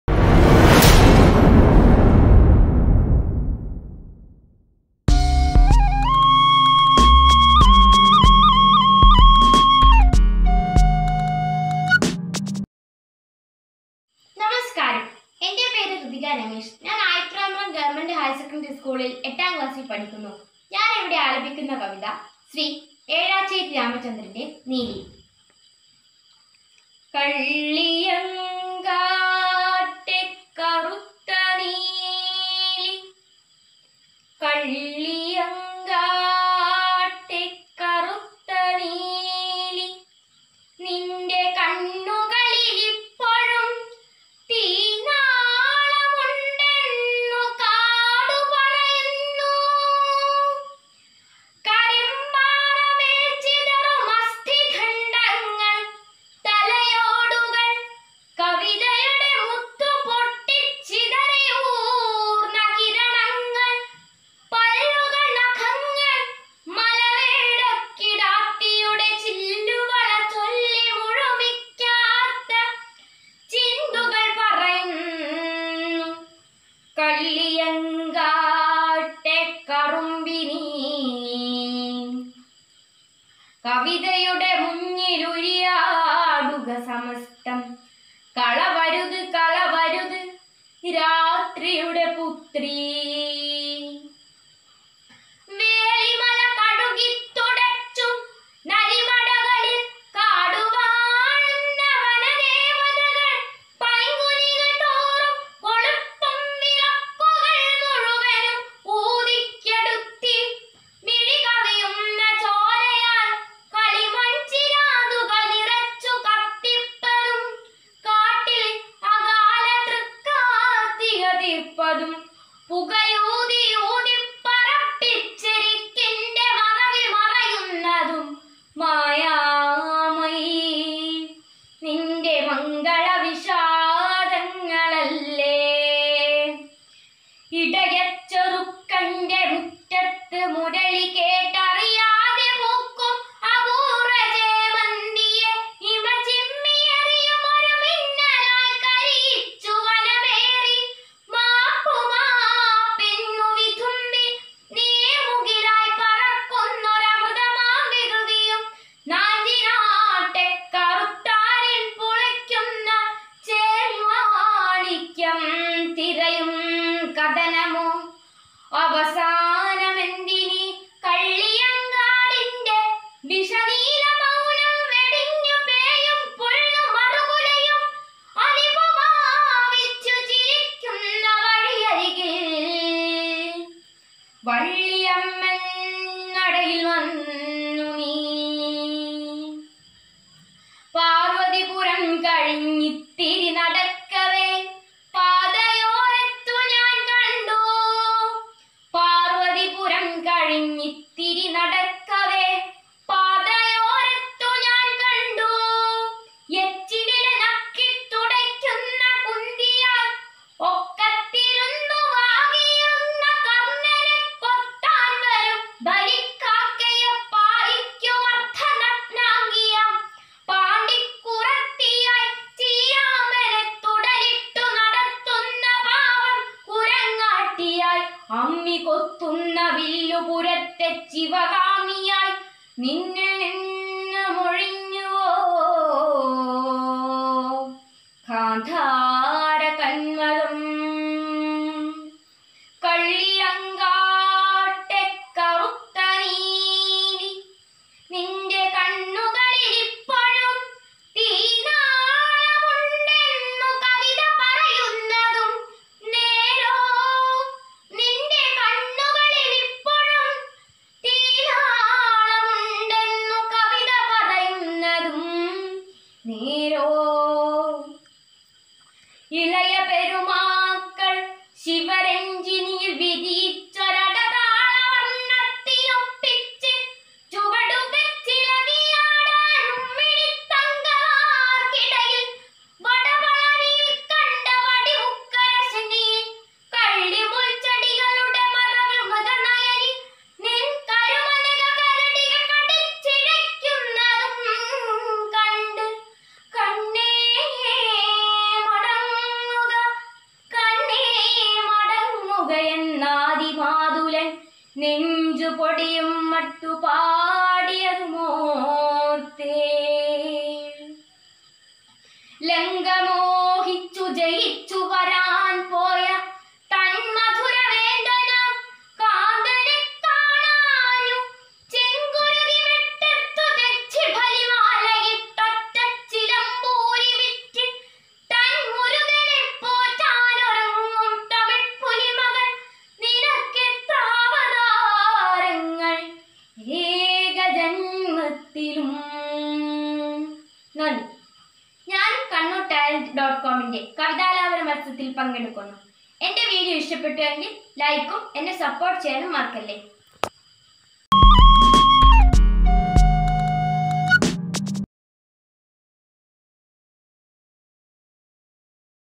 Rithika Ramesh (KT2001-0055) - Ayithara Govt. Higher Secondary School - Kavithaparayanam contest
Rithika Ramesh (KT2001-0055) of Ayithara Govt. Higher Secondary School participate in Kavithaparayanam contest by Kannur Talents